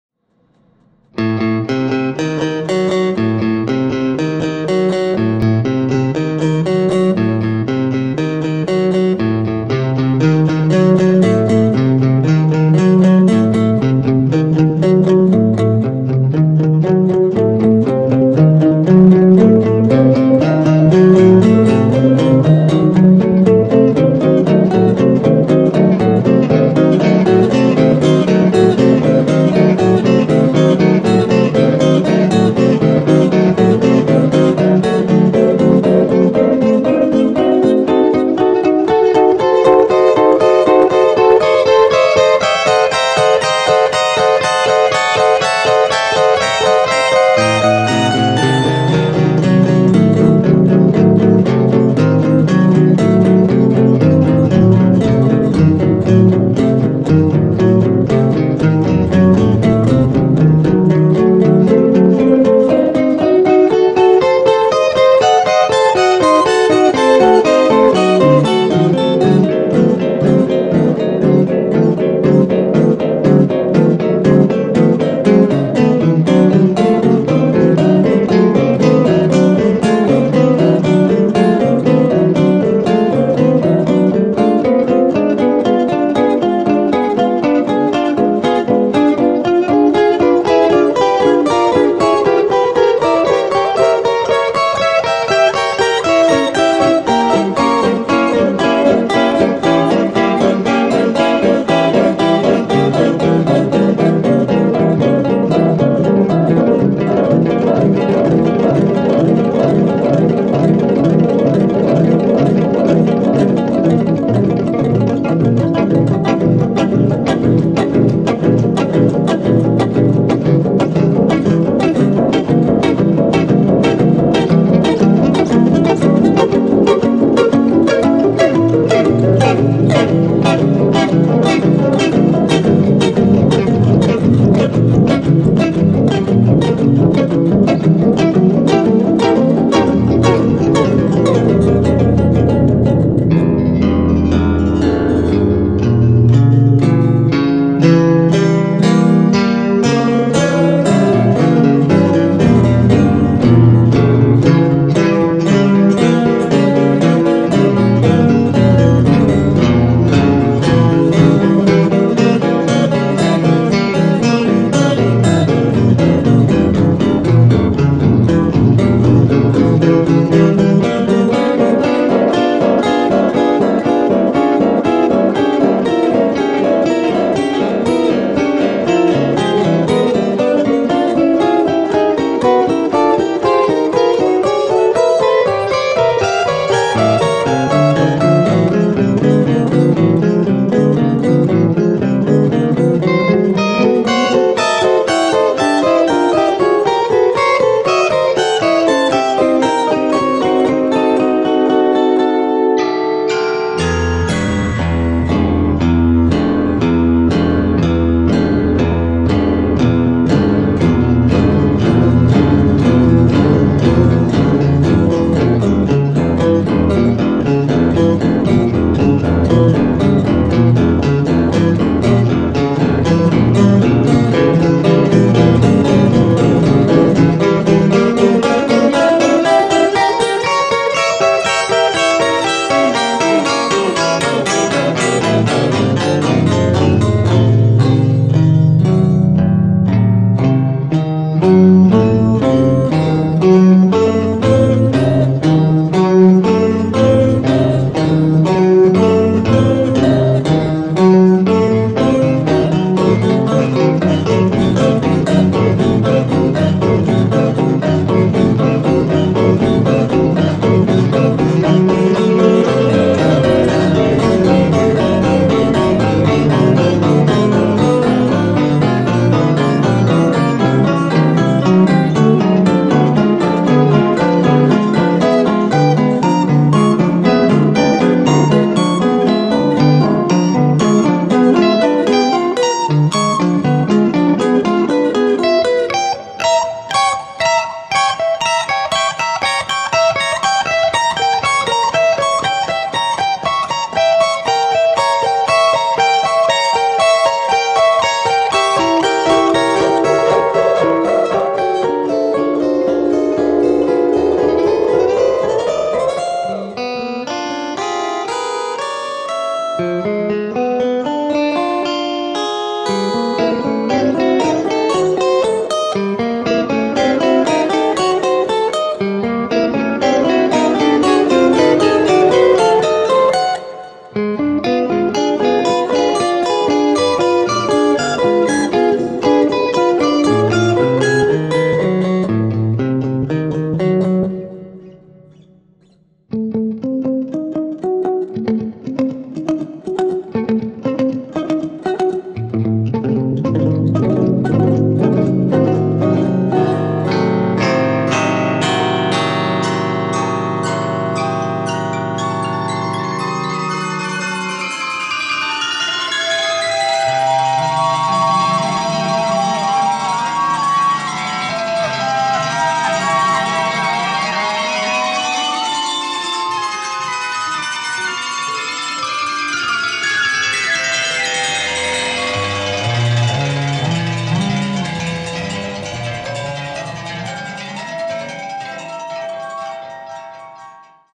Minor Thirds Obsession | Guitar Improvisation
Inspired by Adrian Belew and Robert Fripp

Pitched deleay adds three minor thirds to whatever I play.